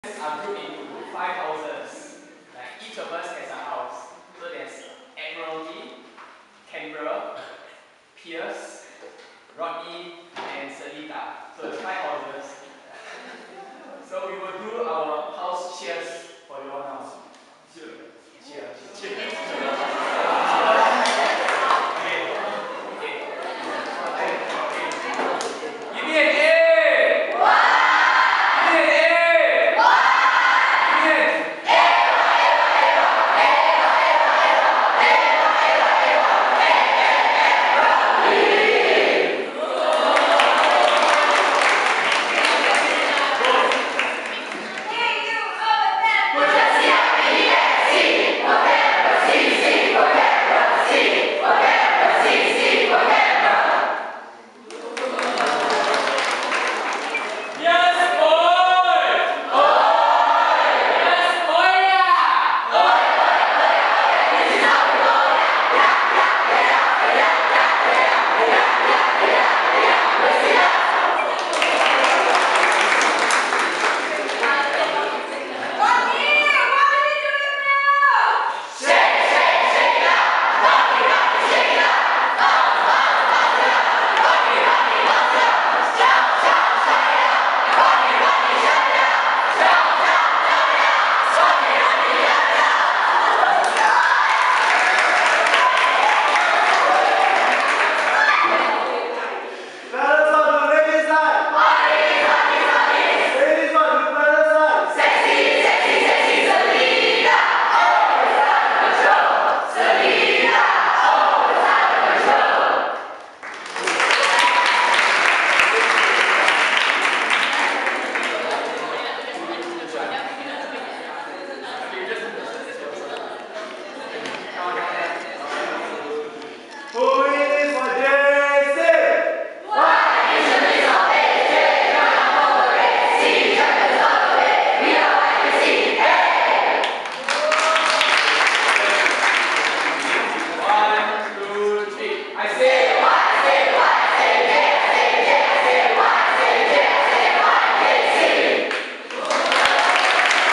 Yishun Junior College Japan Exchange 2015 YJ + House Cheer
Osaka City Senior High School Students Second Batch.
Video Taken by Benseun Kwek Hong Wei on D3100.